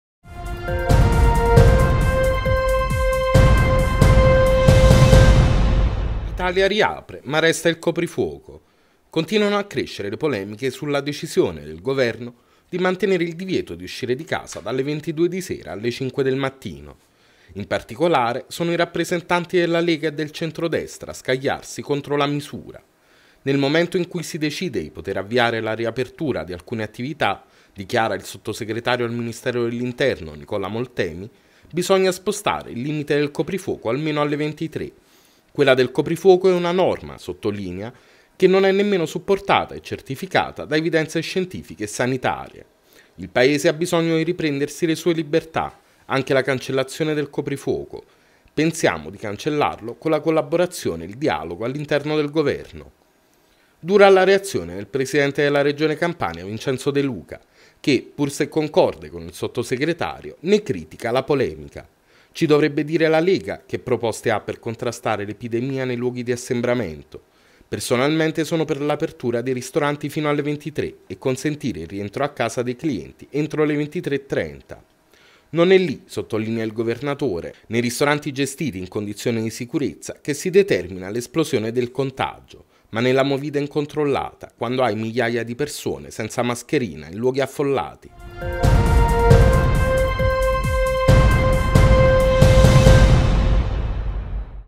Polemiche sul coprifuoco: Lega critica, duro il Governatore De Luca
Italia riapre, ma resta il coprifuoco.
Continuano a crescere le polemiche sul decisione del Governo di mantenere il divieto di uscire di casa dalle 22 di sera alle 5 del mattino.
In particolare, sono i rappresentanti della Lega e del Centro Destra a scagliarsi contro la misura: "Nel momento in cui si decide di poter avviare la riapertura di alcune attività, - dichiara il sottosegretario al Ministero dell’Interno, Nicola Moltemi - bisogna spostare il limite del coprifuoco almeno alle 23. Quella del coprifuoco è una norma - sottolinea - che non è nemmeno supportata e certificata da evidenze scientifiche e sanitarie.
Il Paese ha bisogno di riprendersi le sue libertà, anche la cancellazione del coprifuoco. Pensiamo di cancellarlo con la collaborazione e il dialogo all'interno del Governo".
Dura la reazione del Presidente della Regione Campania Vincenzo De Luca, che, pur se concorde con il sottosegretario, ne critica la polemica: “Ci dovrebbe dire, la Lega, che proposte ha per contrastare l’epidemia nei luoghi di assembramento.
Personalmente sono per l’apertura dei ristoranti fino alle 23 e consentire il rientro a casa dei clienti entro le 23.30.
Non è lì, - sottolinea il Governatore - nei ristoranti gestiti in condizioni di sicurezza, che si determina l’esplosione del contagio, ma nella movida incontrollata, quando hai migliaia di persone senza mascherina in luoghi affollati.”